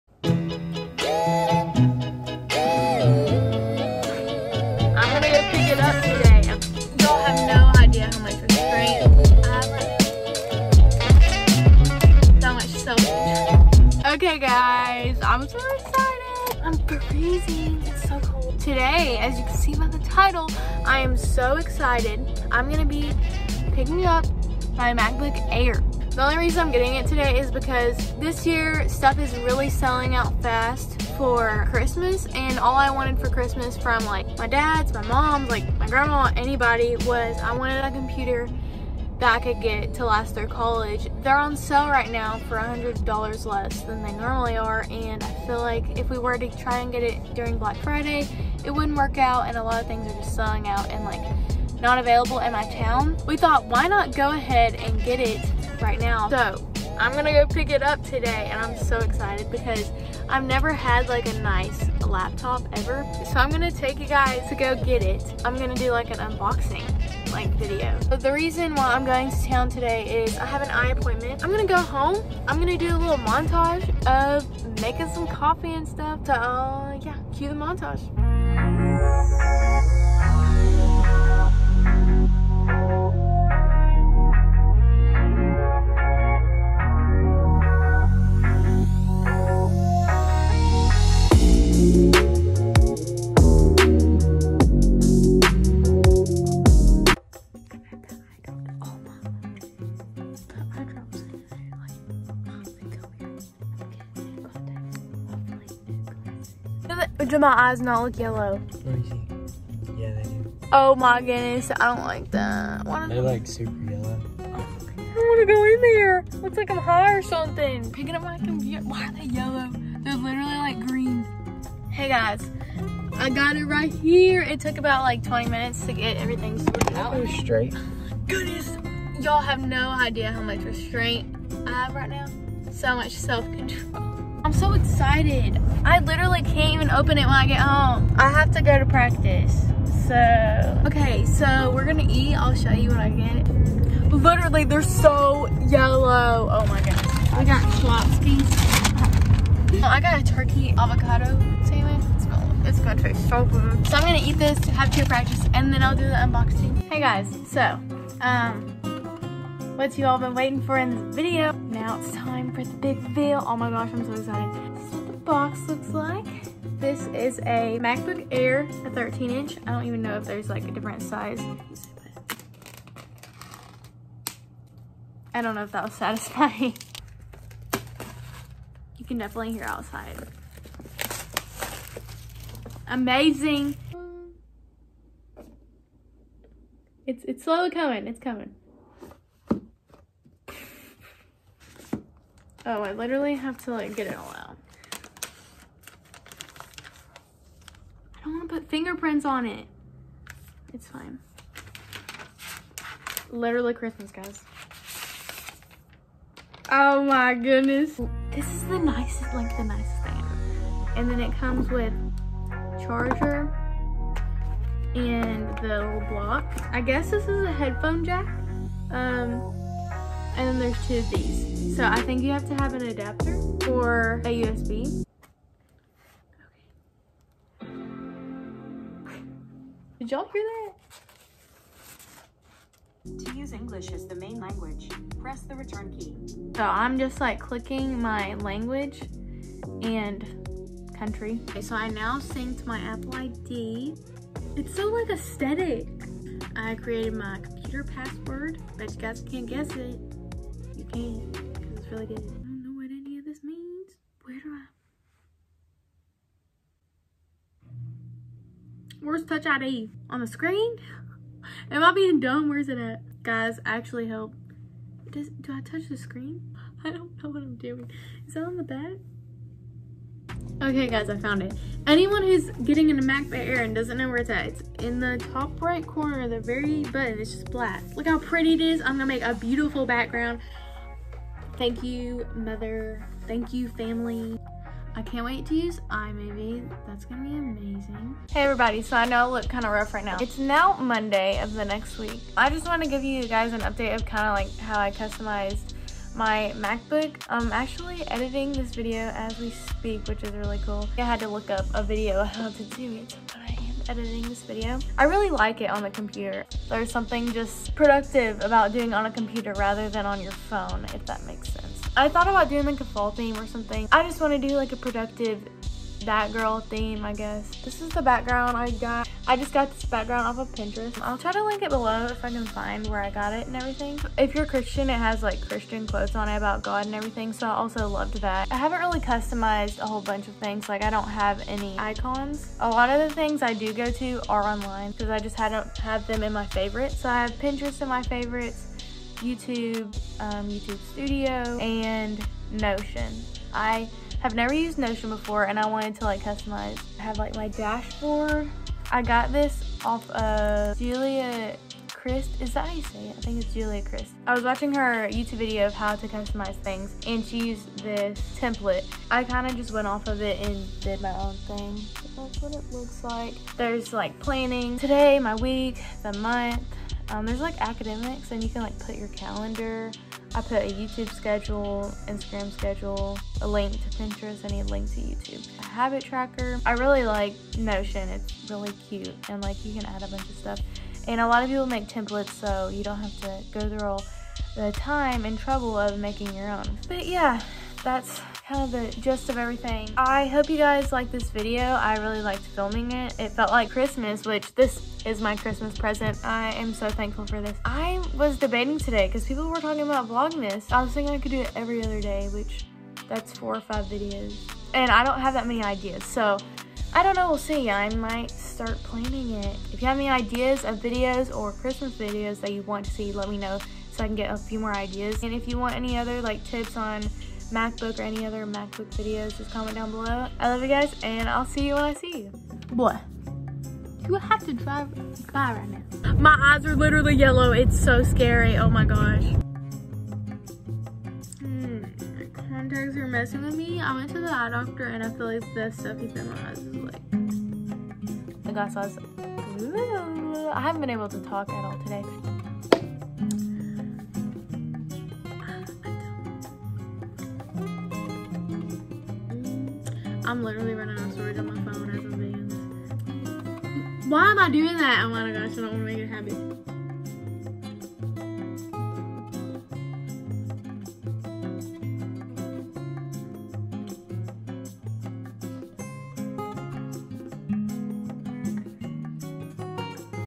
MacBook Air unboxing and setup (customization, organization, asmr)
Hey guys in this video I unbox my MacBook Air! Comment what kind of videos you like. I love to hear your guys feedback.

Comment videos you want to see from me. Have a great video day!